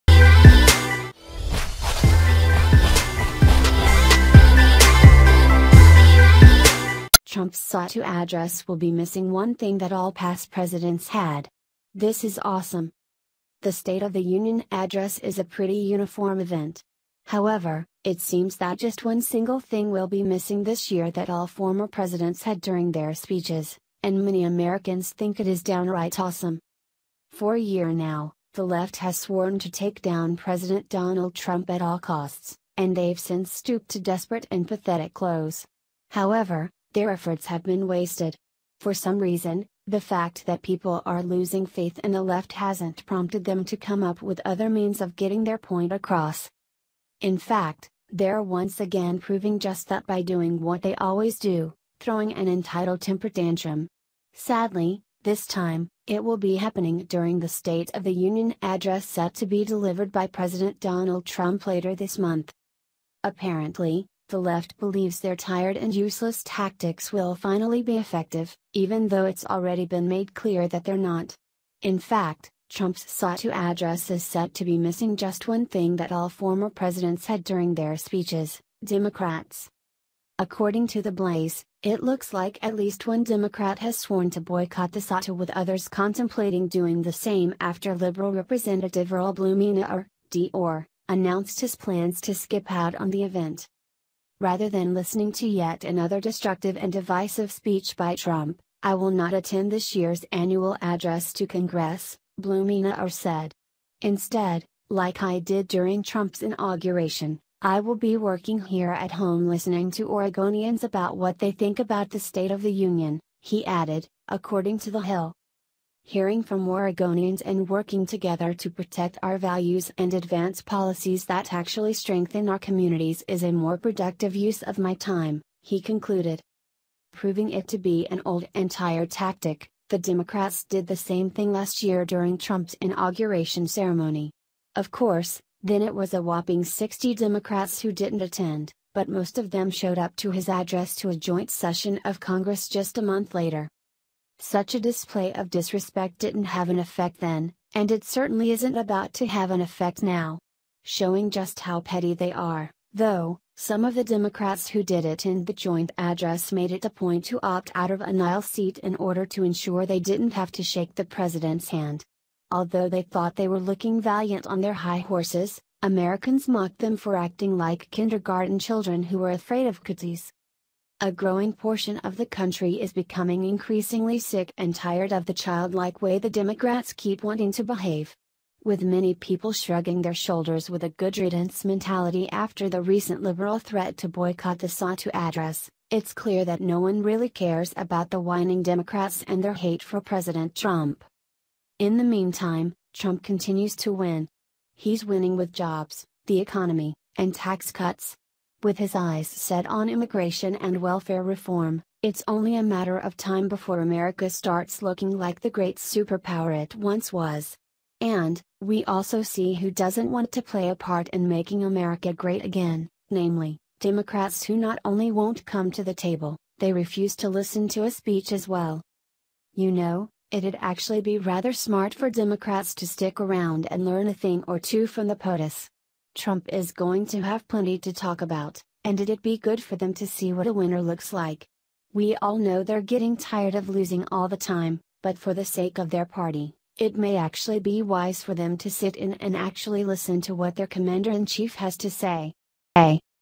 Trump’s SOTU Address Will Be Missing 1 Thing That All Past Presidents Had… This Is AWESOME
The State of the Union address is a pretty uniform event. However, it seems that just one single thing will be missing this year that all former presidents had during their speeches – and many Americans think it is downright awesome.

For a year now, the left has sworn to take down President Donald Trump at all costs, and they’ve since stooped to desperate and pathetic lows. However, their efforts have been wasted. For some reason, the fact that people are losing faith in the left hasn’t prompted them to come up with other means of getting their point across.

In fact, they’re once again proving just that by doing what they always do – throwing an entitled temper tantrum. Sadly, this time, it will be happening during the State of the Union address set to be delivered by President Donald Trump later this month.

Apparently, the left believes their tired and useless tactics will finally be effective, even though it’s already been made clear that they’re not. In fact, Trump’s SOTU address is set to be missing just one thing that all former Presidents had during their speeches – Democrats.
According to The Blaze, it looks like at least one Democrat has sworn to boycott the SOTU with others contemplating doing the same after liberal Rep. Earl Blumenauer (D-Ore.) announced his plans to skip out on the event.

“Rather than listening to yet another destructive and divisive speech by Trump, I will not attend this year’s annual address to Congress,” Blumenauer said. “Instead, like I did during Trump’s Inauguration, I will be working here at home listening to Oregonians about what they think about the State of the Union,” he added, according to The Hill.

“Hearing from Oregonians and working together to protect our values and advance policies that actually strengthen our communities is a more productive use of my time,” he concluded.
Proving it to be an old and tired tactic, the Democrats did the same thing last year during Trump’s inauguration ceremony. Of course, then it was a whopping 60 Democrats who didn’t attend, but most of them showed up to his address to a joint session of Congress just a month later.

Such a display of disrespect didn’t have an effect then, and it certainly isn’t about to have an effect now. Showing just how petty they are, though, some of the Democrats who did attend the joint address made it a point to opt out of an aisle seat in order to ensure they didn’t have to shake the president’s hand. Although they thought they were looking valiant on their high horses, Americans mocked them for acting like kindergarten children who were afraid of “cooties.”

A growing portion of the country is becoming increasingly sick and tired of the childlike way the Democrats keep wanting to behave. With many people shrugging their shoulders with a “good riddance” mentality after the recent liberal threat to boycott the SOTU address, it’s clear that no one really cares about the whining Democrats and their hate for President Trump.

In the meantime, Trump continues to win. He’s winning with jobs, the economy, and tax cuts. With his eyes set on immigration and welfare reform, it’s only a matter of time before America starts looking like the great superpower it once was. And, we also see who doesn’t want to play a part in making America great again; namely, Democrats who not only won’t come to the table, they refuse to listen to a speech as well.
You know, it’d actually be rather smart for Democrats to stick around and learn a thing or two from the POTUS. Trump is going to have plenty to talk about, and it’d be good for them to see what a winner looks like. We all know they’re getting tired of losing all the time, but for the sake of their party, it may actually be wise for them to sit in and actually listen to what their Commander-in-Chief has to say.